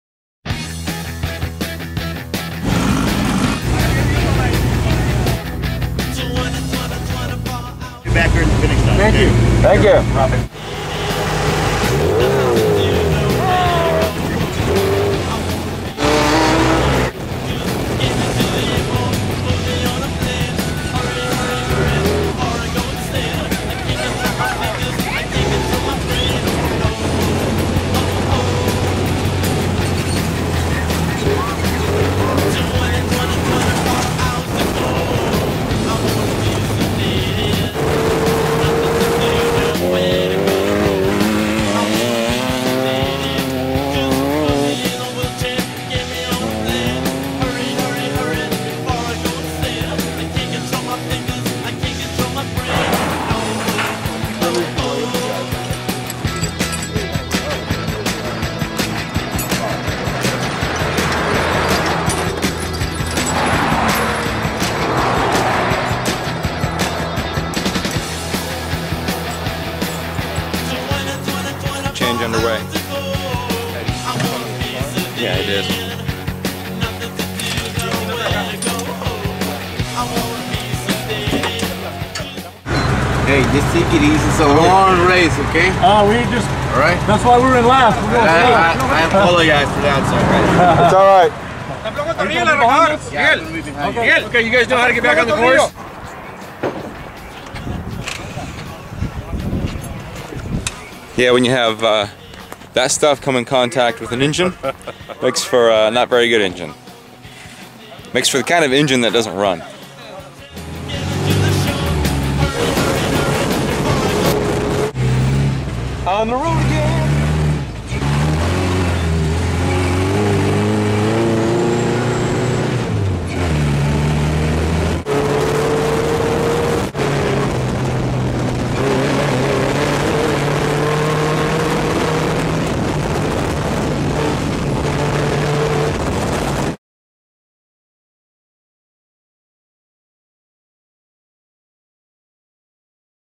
33 Minute Engine Change!! 2008 Baja 1000
TEAM CarbonCopyPRO blows an engine. Watch it get changed in 33 minutes!